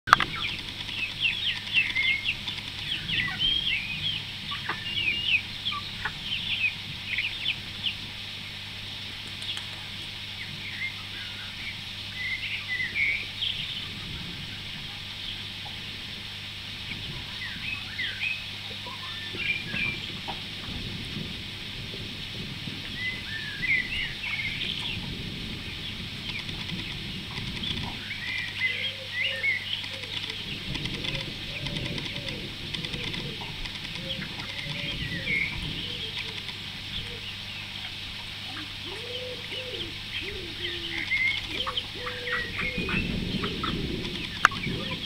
Schröder und die Chicken ...
Schröder läßt sich sogar seinen Knochen von den Küken klauen ...